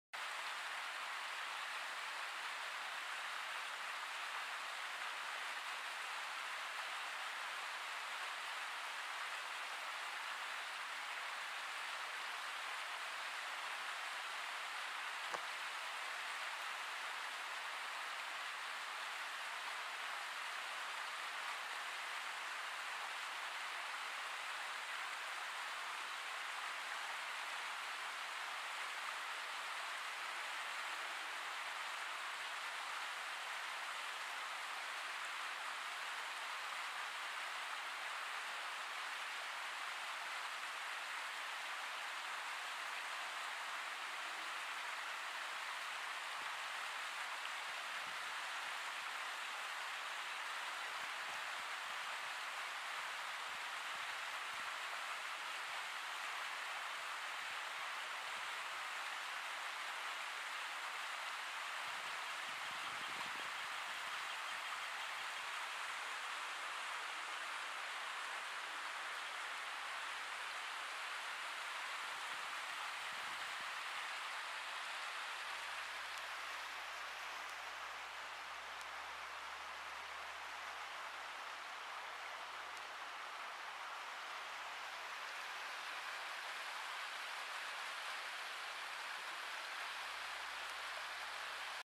Ducks Relaxing Behind the Barn
Thursday, May 6, 2021, 5:25 PM Sainte-Adèle, Quebec, Canada
Weather Network report at the time: 13°C. Partly cloudy. Feels like 11